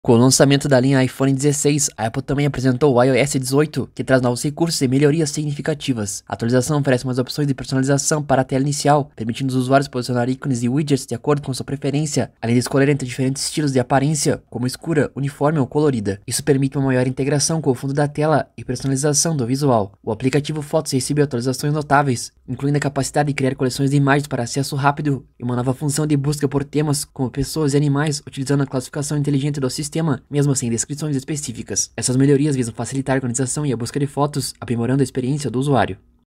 Com o lançamento da linha iPhone 16, a Apple revelou o iOS 18 oficialmente
Com o lançamento da linha iPhone 16, a Apple também apresentou o iOS 18, trazendo novas funcionalidades de personalização e melhorias em aplicativos.